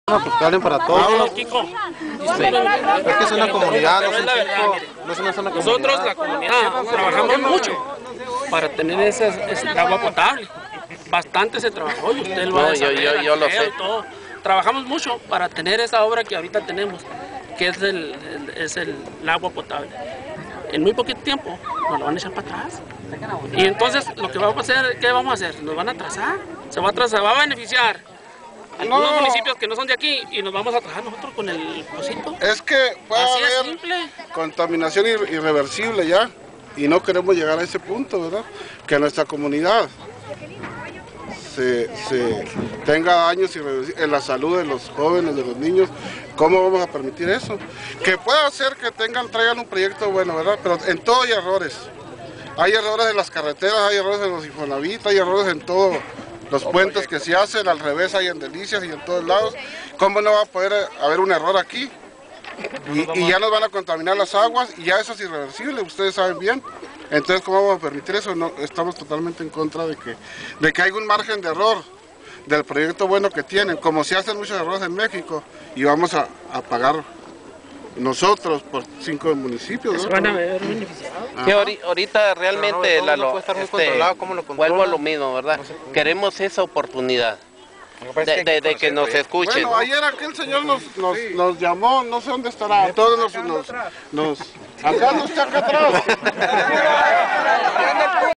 Manifestacion en Los Jaquez
Ciudadanos de los Jaquez se manifiestan en contra de la construcción del relleno sanitrio regional en la comunidad